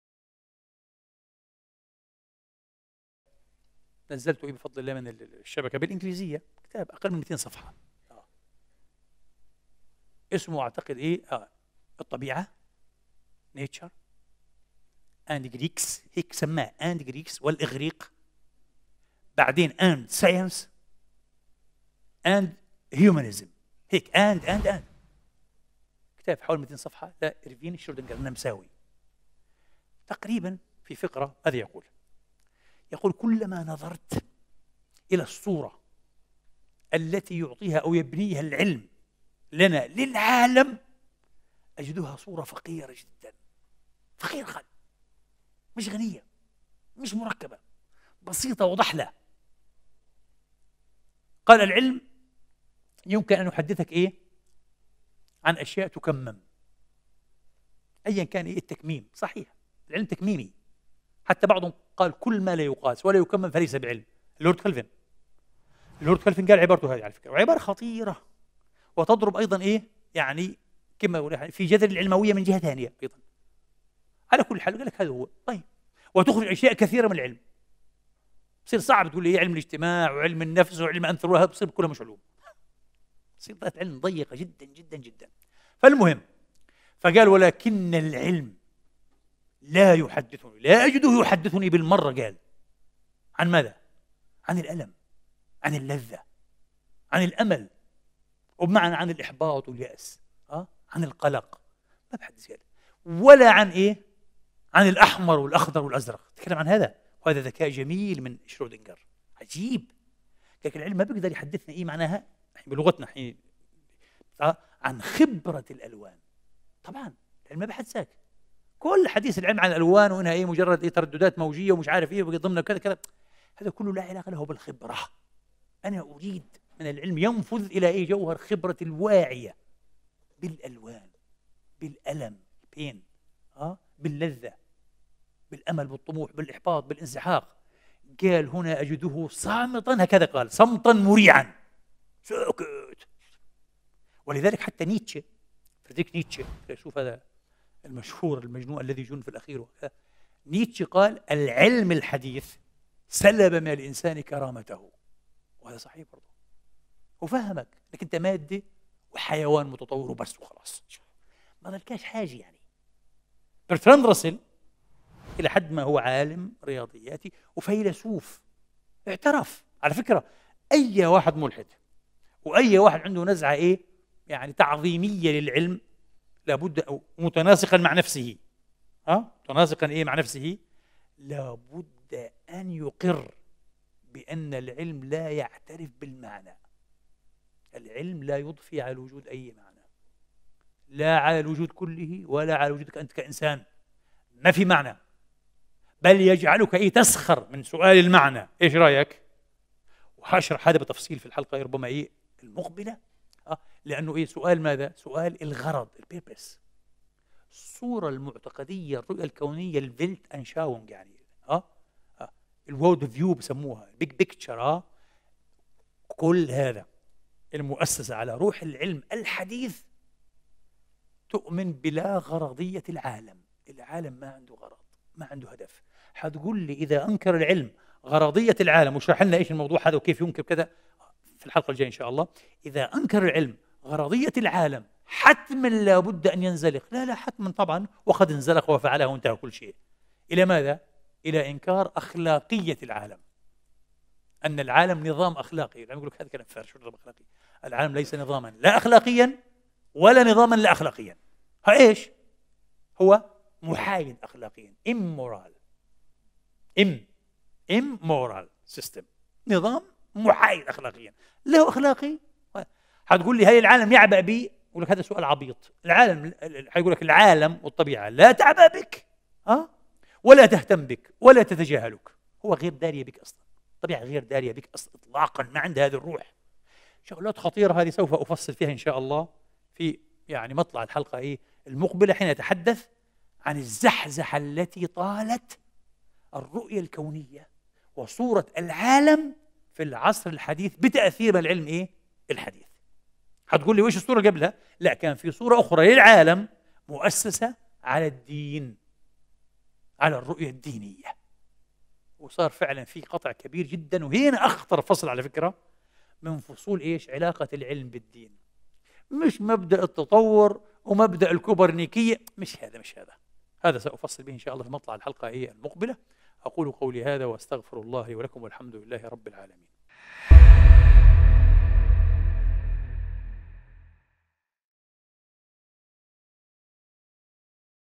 شرودنغر رأى أن العلم لا يحدثه عن هذه المشاعر وأن صورة العالم به وحده فقيرة وبلا معنى! عدنان إبراهيم
من الحلقة السادسة؛ نموذج الانفصال ومجاز الكتابين، من سلسلة العلم والدين - أينشتاين وسندريلا، للدكتور عدنان إبراهيم

للوصول إلى حسابات فضيلته الرسمية:
adnanibrahim#
adnan_ibrahim#